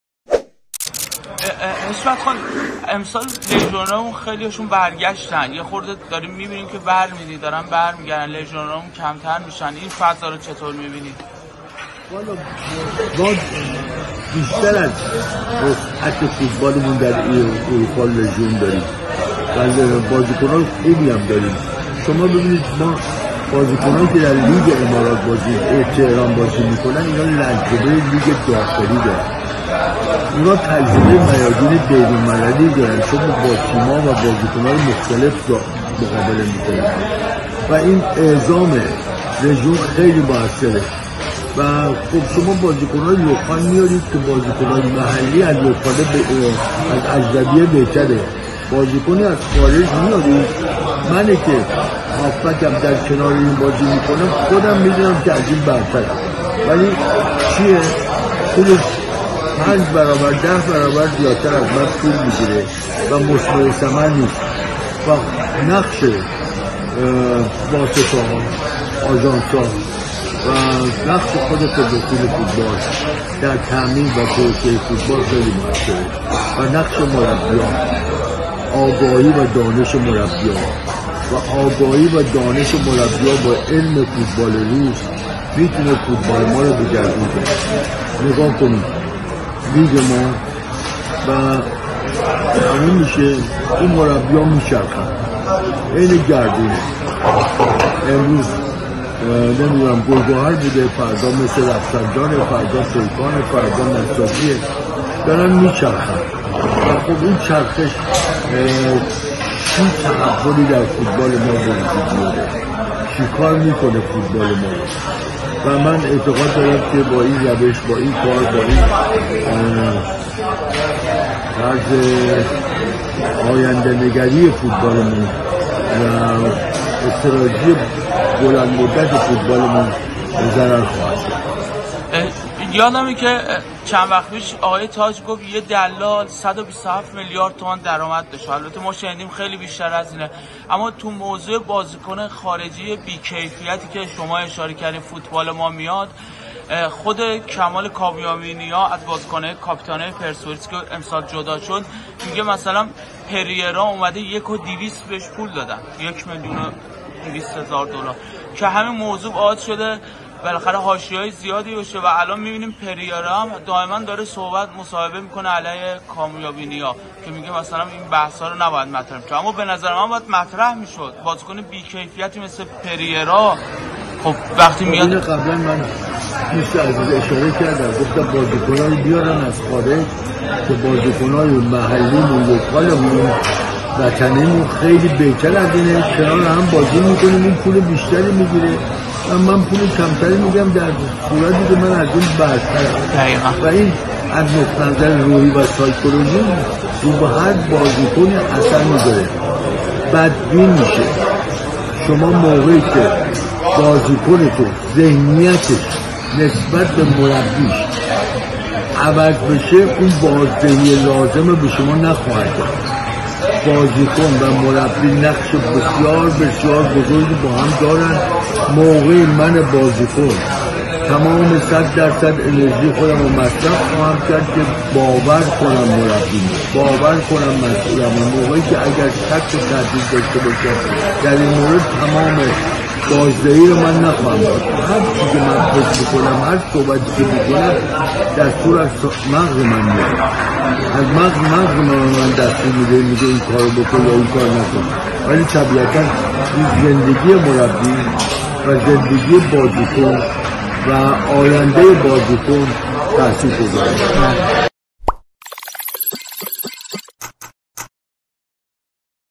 بیشتر از وسعت فوتبالمان در اروپا لژیونر داریم
حشمت مهاجرانی در گفتگو با همشهری از اوضاع و احوال فوتبال ایران می گوید